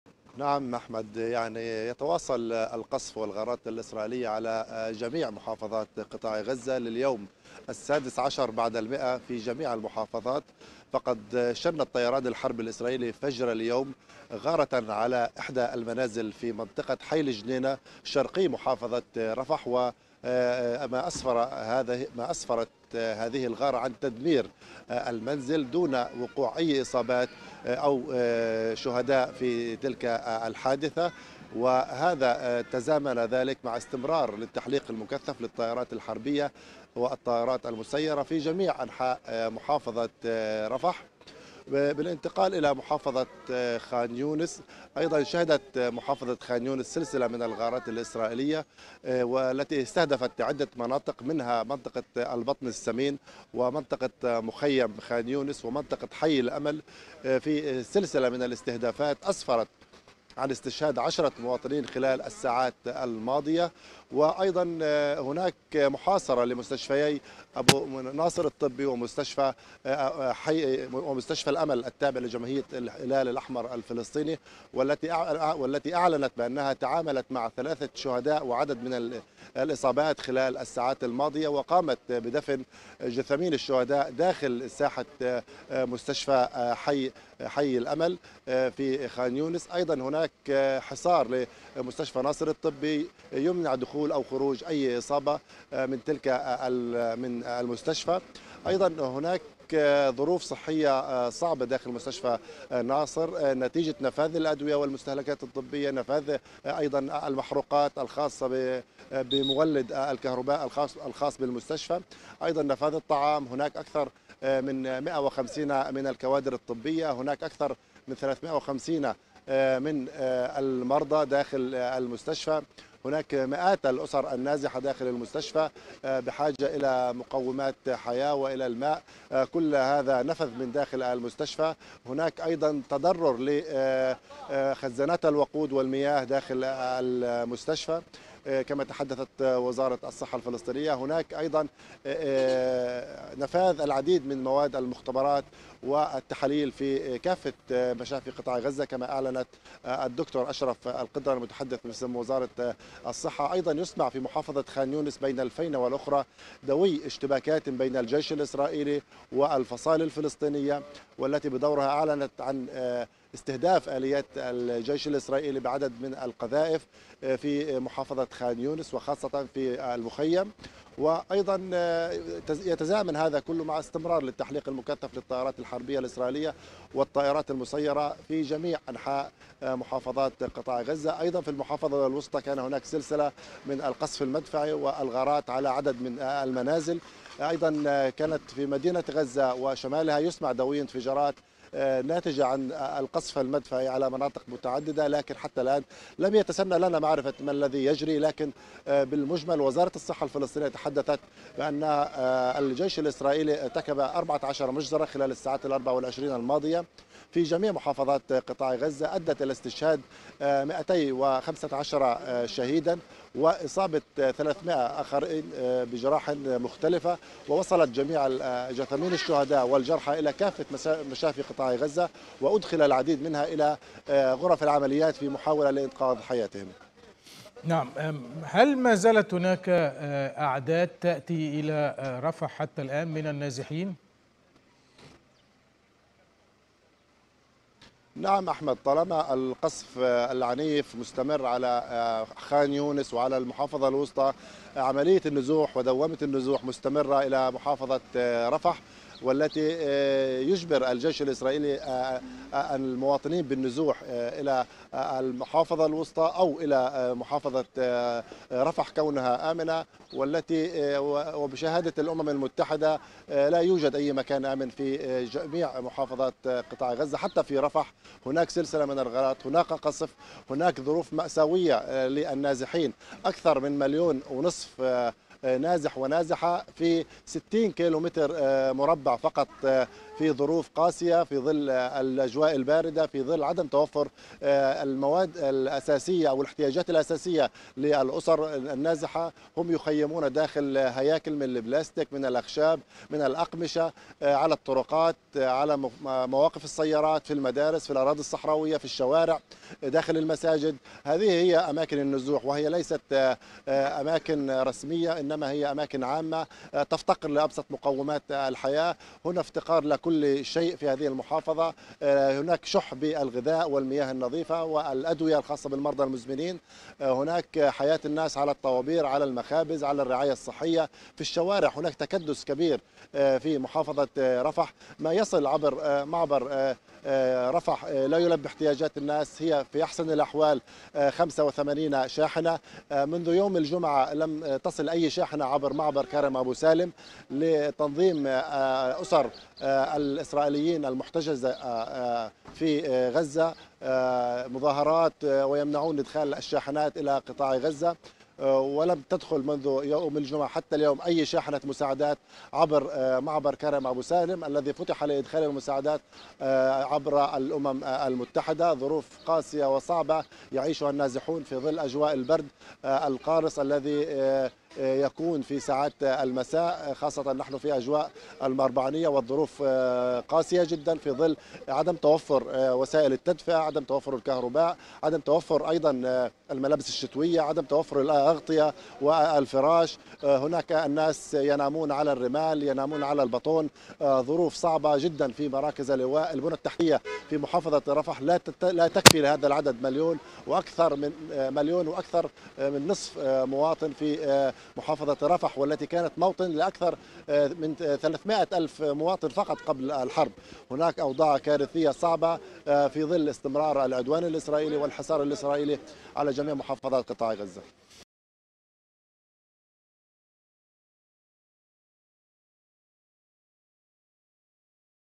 10 شهداء في سلسلة استهدافات إسرائيلية في خان يونس
عشرون شهيدا على الأقل في قصف للاحتلال على حي الصبرة بمدينة غزة.. وأكثر من مائتي شهيد وثلاثمئة مصاب خلال 24 ساعة.. وارتفاع عدد شهداء العدوان الإسرائيلي على القطاع لأكثر من ستة وعشرين ألفا وستمئة.. وحماس والجبهة الشعبية تشترطان وقف العدوان وسحب قوات الاحتلال من غزة قبل أي صفقة للتبادل.. ونتنياهو يتحدث عن شروط غير مقبولة لإسرائيل.. وقطر تشير إلى تقدم في المحادثات.. وواشنطن تؤكد الحاجة للكثير من العمل للتوصل لاتفاق.. وقوة إسرائيلية خاصة تغتال ثلاثة شبان فلسطينيين داخل مستشفى ابن سينا في جنين.. وجيش الاحتلال يحاصر مخيم نور شمس شرقي طولكرم وسط تدمير واسع للبنية التحتية.. وتقارير أميركية تكشف عن طلب بايدن من مستشاريه خيارات للرد بعد استهداف قاعدة أميركية في الأردن, وأوستن يتعهد باتخاذ كل الإجراءات الضرورية.. وبلينكن يقول إن الرد على إيران قد يكون متعدد المستويات وعلى مراحل.
المزيد من التفاصيل يتحدث مراسل الغد من رفح أيمن أبو شنب مع الإعلامي أحمد بصيلة عبر الفقرة الإخبارية.